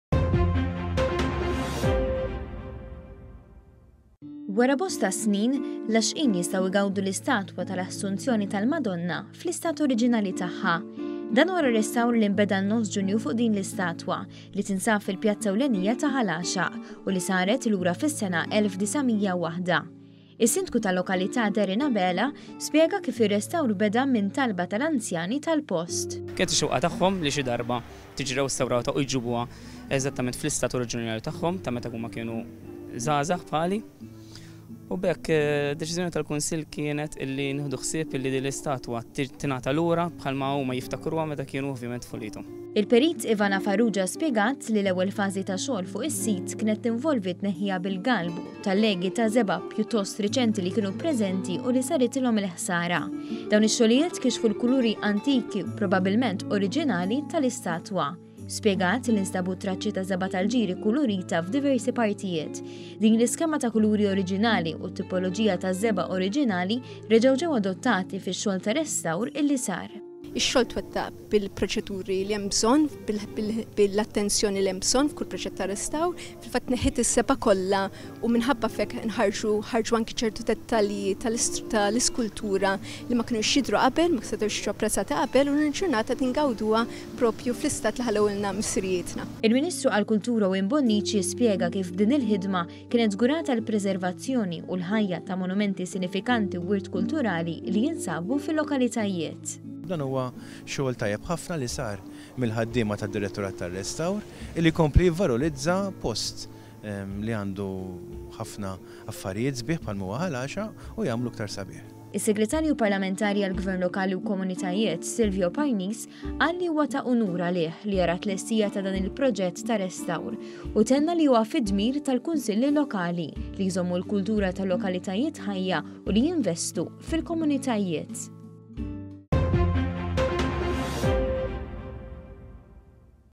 JITLESTA R-RESTAWR FUQ L-ISTATWA TAL-ASSUNZJONI F’ĦAL GĦAXAQ..
ONE.COM.MT - Tlesta r-restawr fuq l-istatwa tal-Assunzjoni tal-Madonna f’Ħal Għaxaq, biex wara għexieren ta’ snin, tista’ titgawda minn kulħadd fl-istat oriġinali tagħha.